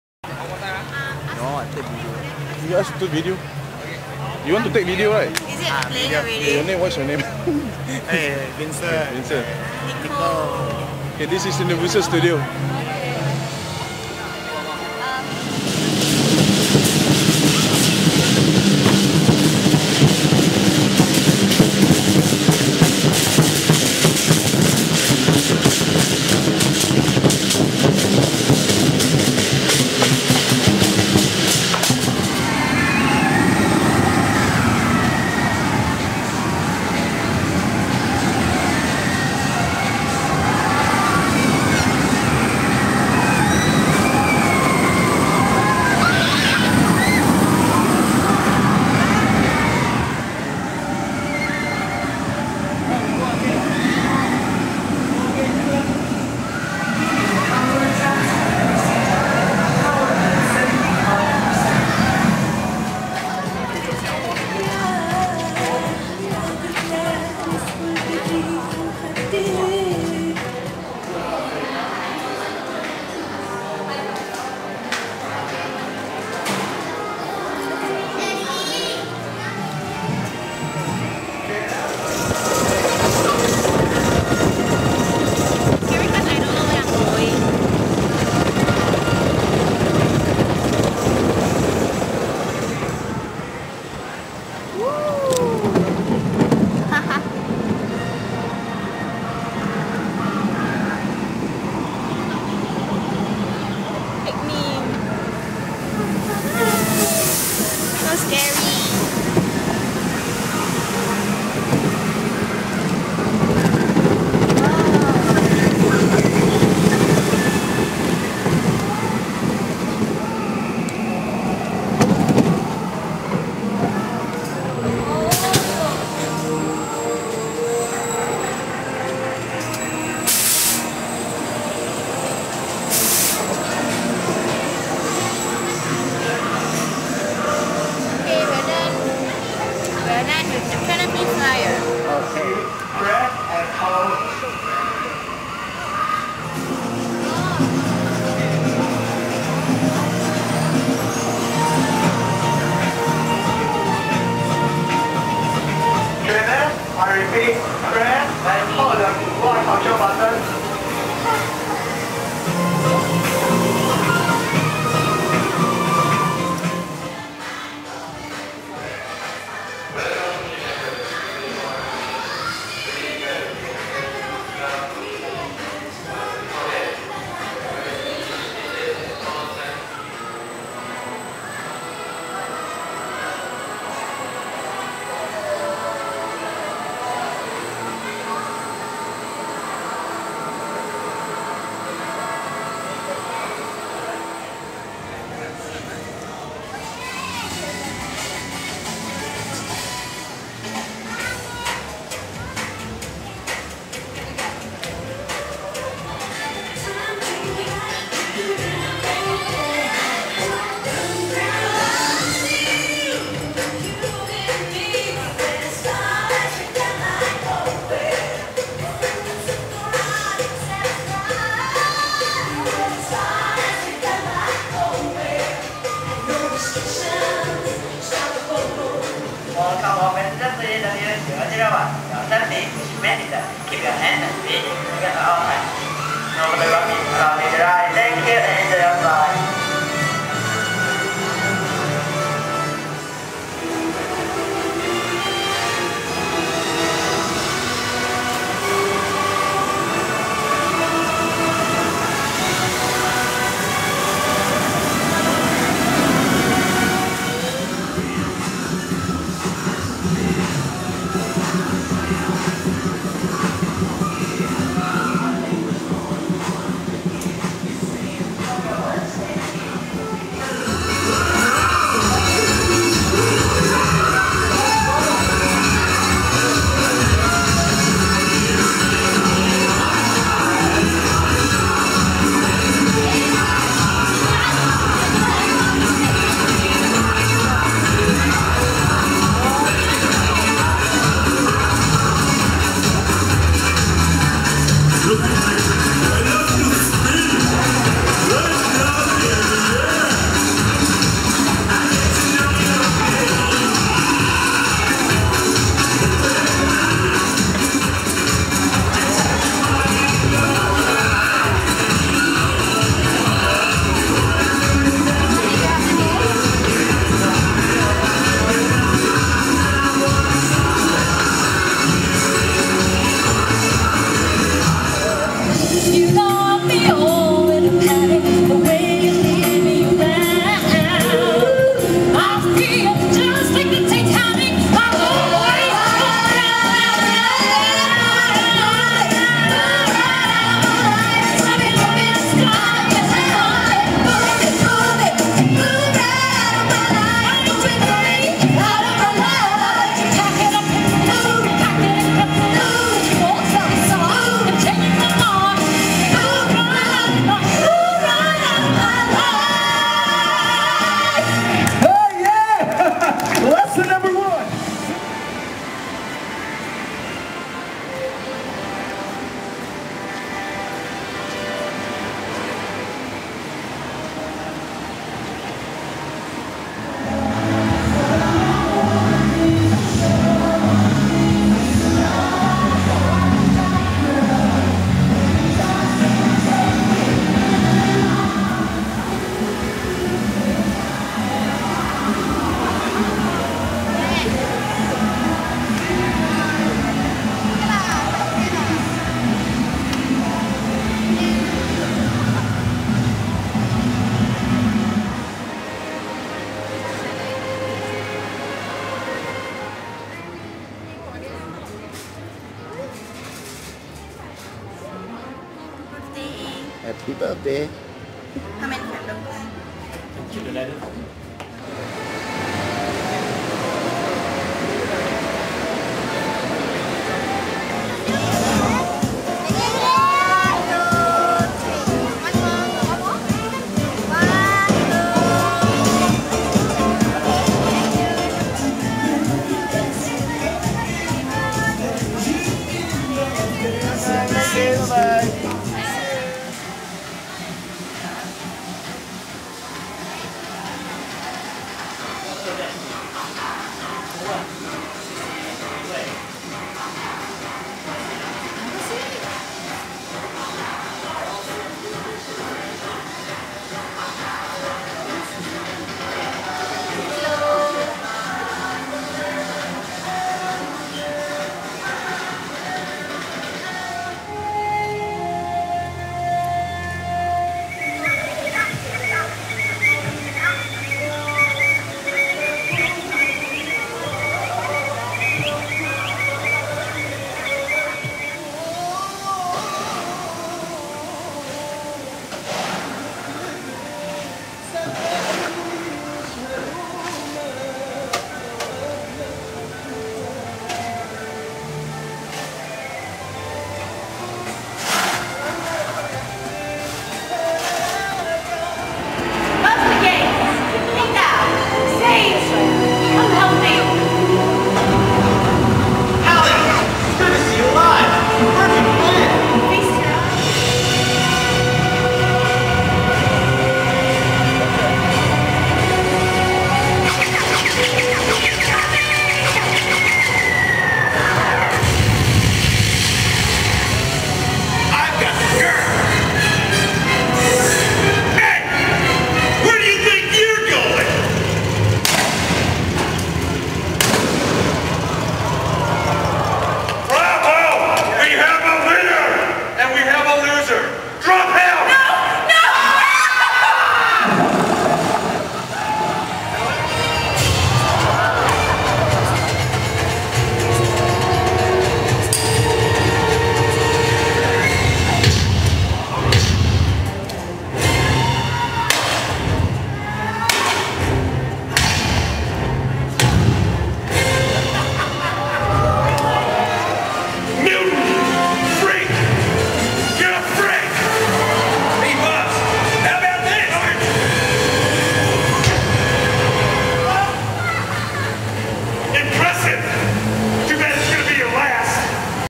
Universal Studios Singapore Resorts World Sentosa Opening Day-Vincent-Lucky Man In Singapore
Vincent-The Lucky Man In Singapore and his youngest daughter was at the soft opening of Universal Studios Singapore at Resorts World Sentosa (RWS) on Thursday, March 18 2010.

A very limited number of tickets were sold to avoid over-stressing the system and so there were hardly any queues at the 18 rides.

Everything was beautiful and the rides were really fun and most of us gave the theme park two thumbs-up.

This 20-hectare park in Singapore is Universal Studios' fourth after those in Orlando, Hollywood and Osaka.

for more articles about Singapore.